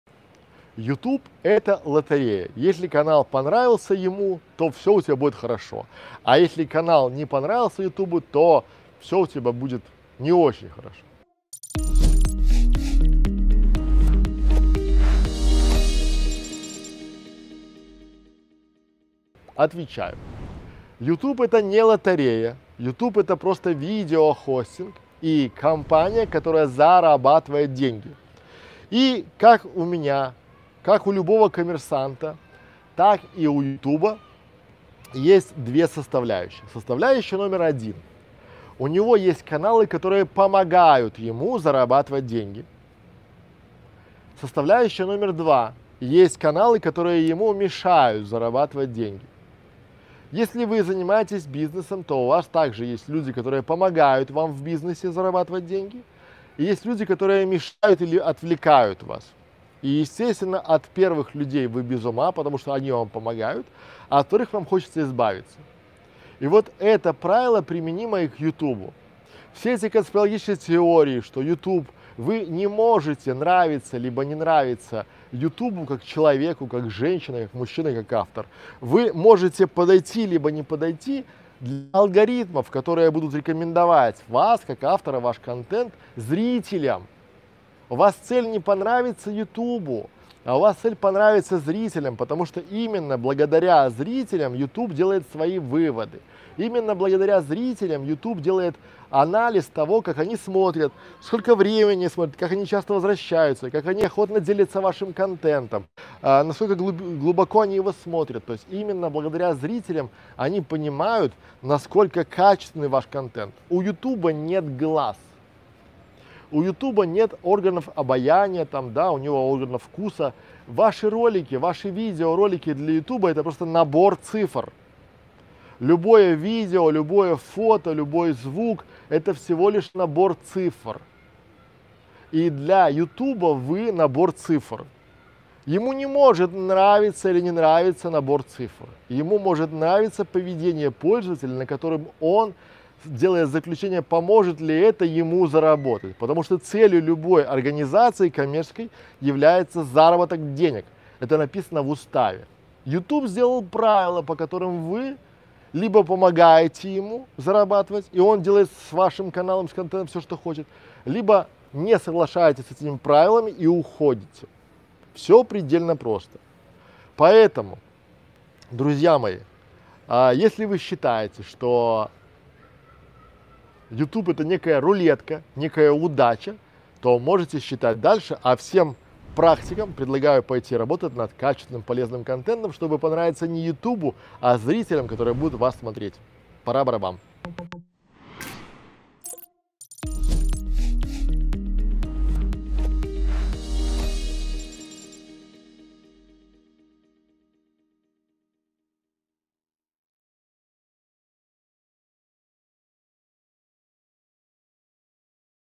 Заработок на YouTube канале - это лотерея? Как понравиться ютубу?
Действительно ли возможность зарабатывать на YouTube можно считать лотереей? Какие 2 составляющие есть у любого бизнеса? Смотрите это видео и Вы узнаете, как именно YouTube понимает необходимость Вашего контента. Существуют ли какие-то критерии, по которым Ваш канал будет оценивать YouTube?

Переходите на канал Бесплатной Школы Видеоблогера, чтобы регулярно повышать свои скиллы по работе над своим видеоконтентом.